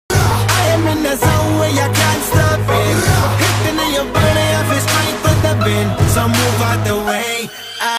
BBQ Platter | Frontier Karahi Boat Basin Karachi | Dr Foodie Official
Best BBQ Platter in Karachi is offered by Frontier Karahi located at Boat Basin Karachi.
Their prices are also economical.

If you like my videos kindly do comment and press the Like button.